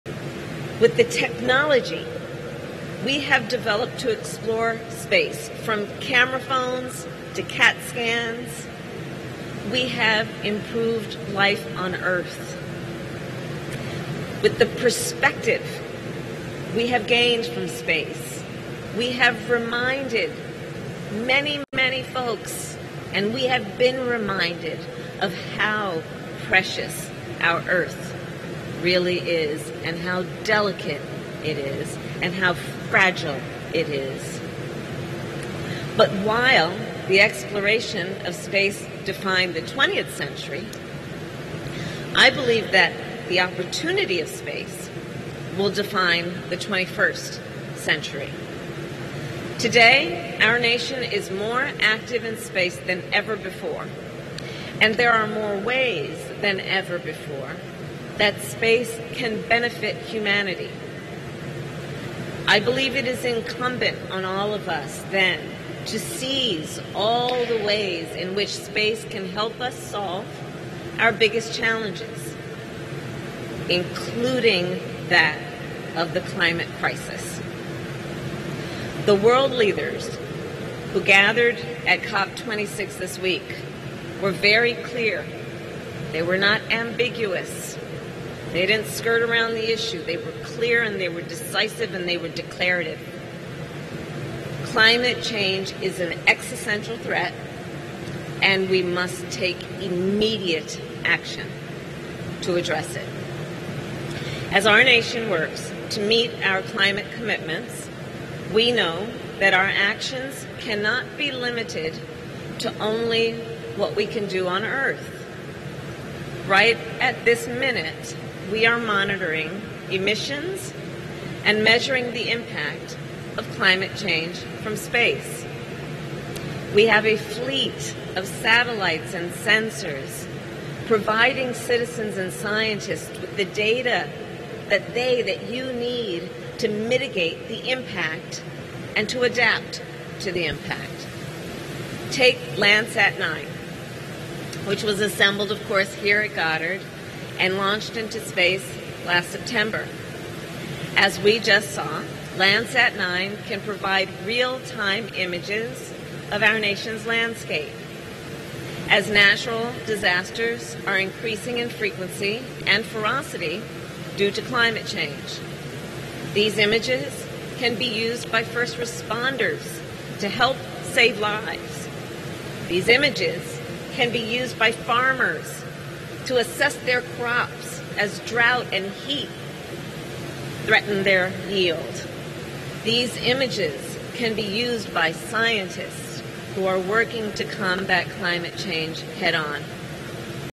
VP Harris Touts Work Of NASA As Instrumental In Fighting Climate Change
VP Kamala Harris speaks at the NASA Goddard Space Center on Friday.